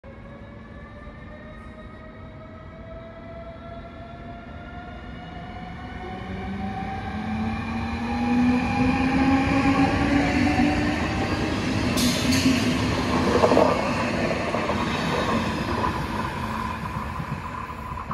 Ausfahrt Flixtrain (sehr kurz!) + RailPool Vectron
Eine RailPool Vectron (Br 193) zieht einen sehr kurzen Flixtrain in Richtung Stuttgart.

A RailPool Vectron (German Class 193) pulls a Flixtrain towards Stuttgart.